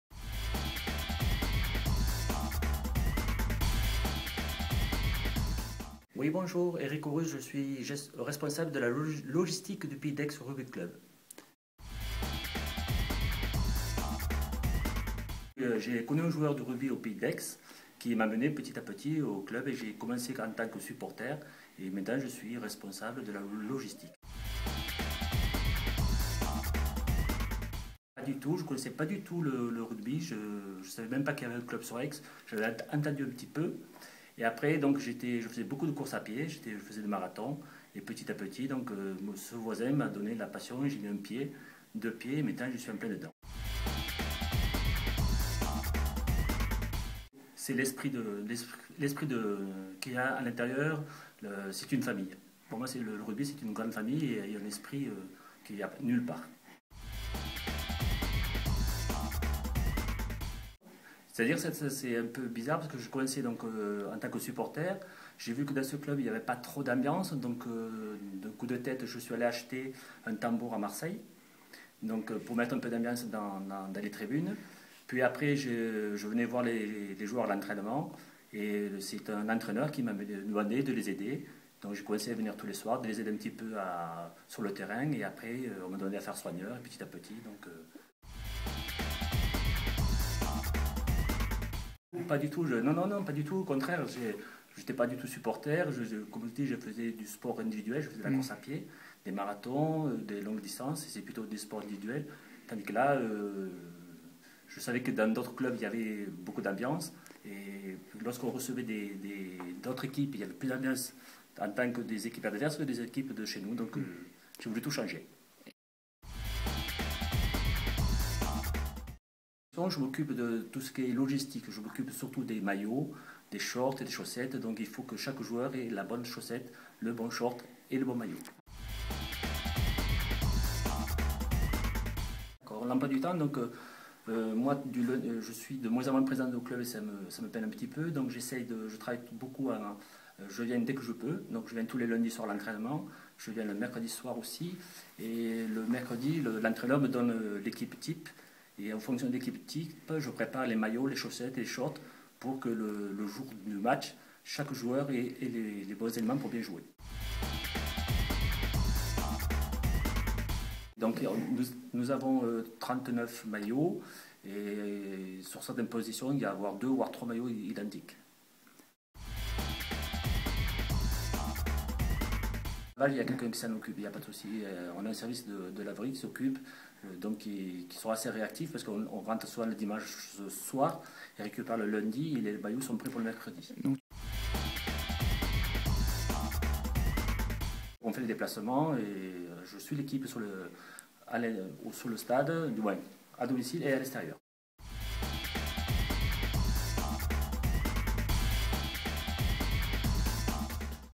La belle histoire du Pays d'Aix Rugby Club : entretien avec Eric Aurus
Tout club a de belles histoires sympathiques à raconter, le PARC en fait partie. Entretien avec Eric Aurus, chargé logistique pour l'équipe première.